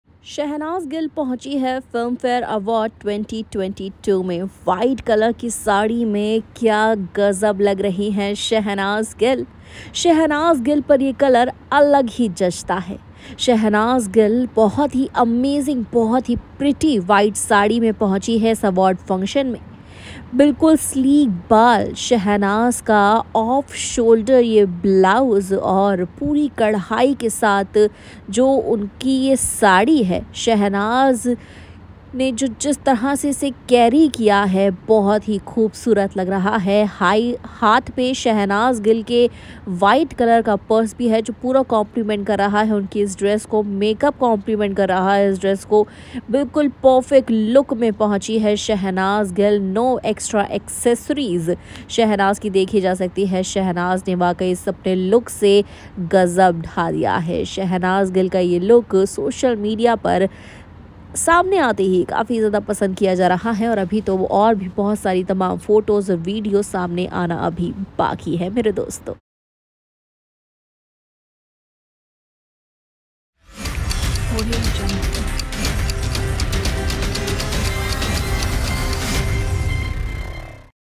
Shehnaaz Gill Looking Gorgeous in White Saree at Filmfare Awards 2022 | 67th Filmfare Awards
Shehnaaz Gill Looking Beautiful in White Saree at 67th Filmfare Awards 2022 |